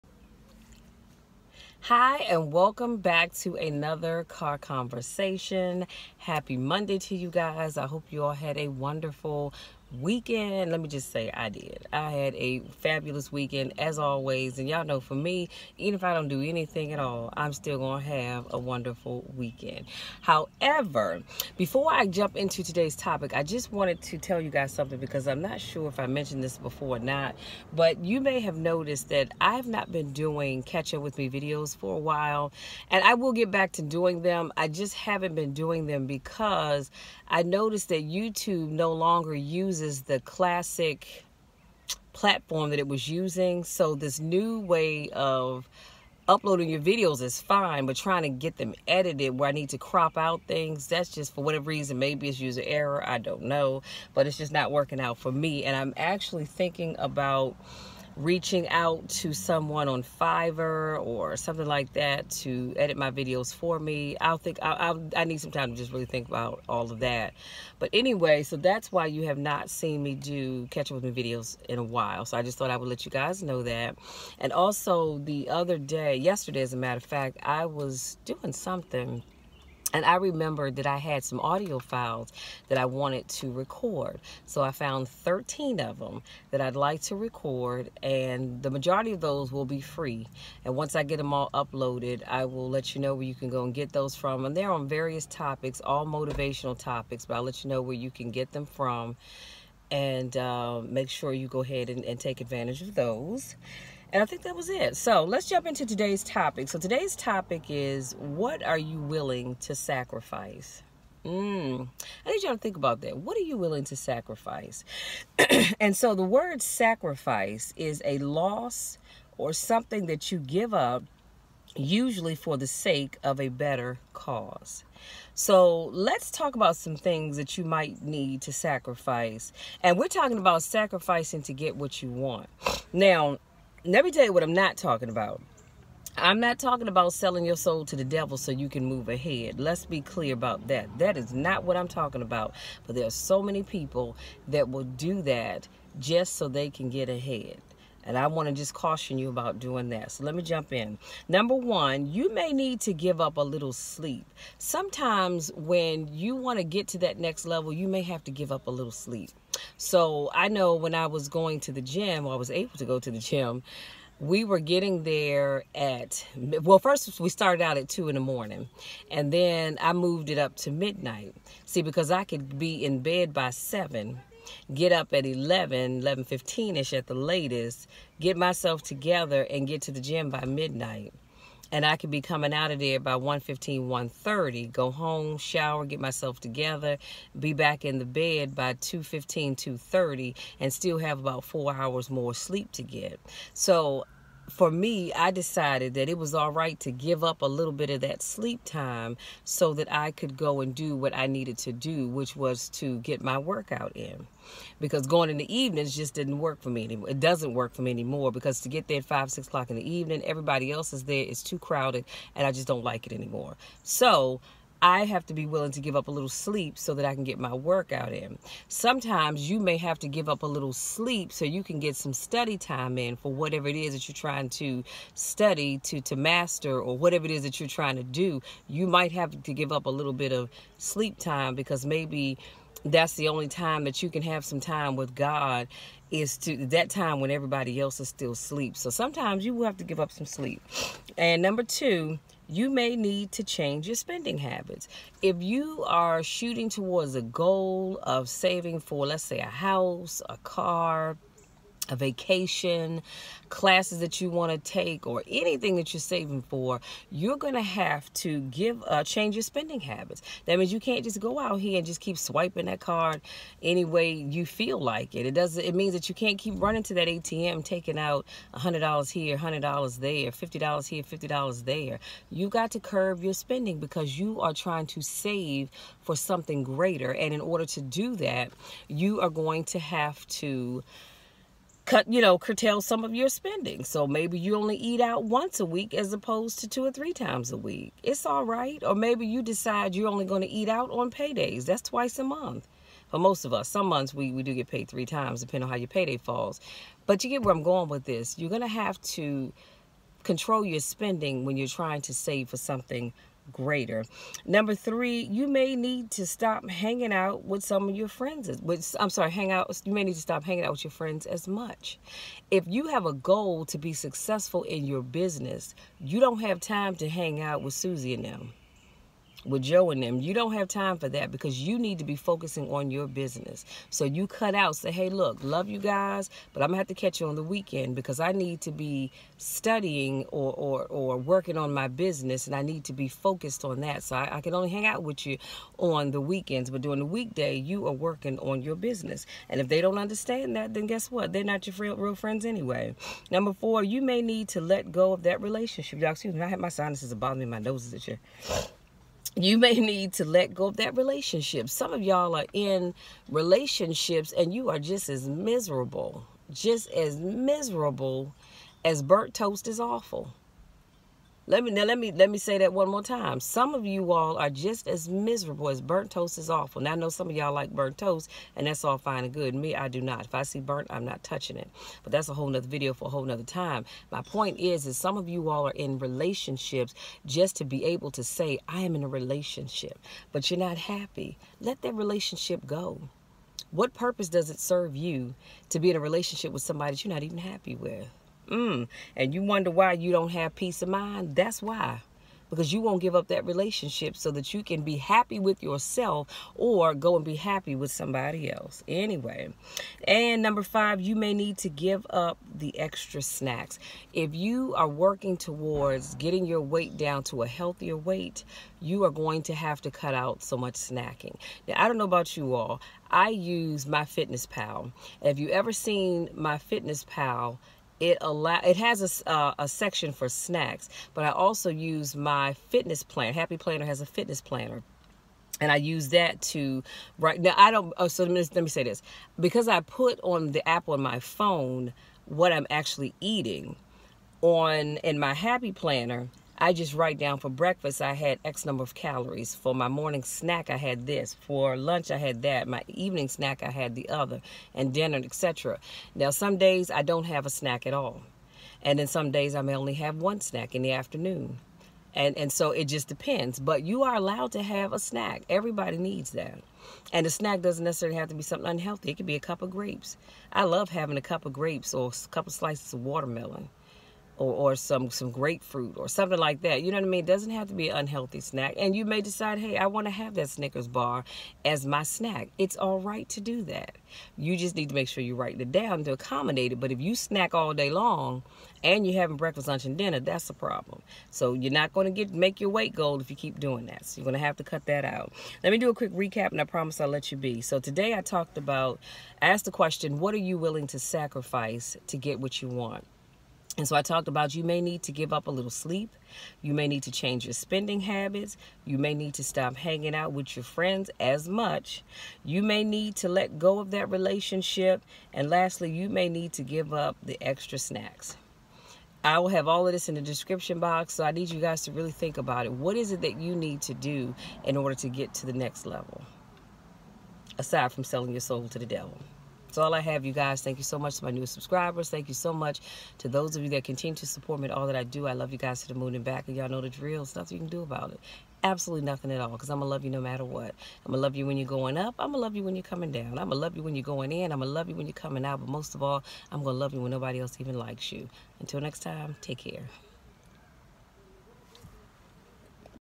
What Are You Willing To Sacrifice
Hi and welcome back to another Car Conversation! Todays topic is all about giving up something, in order to get something better. Here are somethings you may need to give up:

1. You my need to give up some sleep, in order to spend time on something you love.

2. You my need to change your spending habits.

3. You may need to stop hanging out with your friends as much and focus more time on you/ and your business.

4. You may need to let go of that relationship, especially if it's toxic.

5.  You may need to give up the extra snacks, if you are trying to lose weight.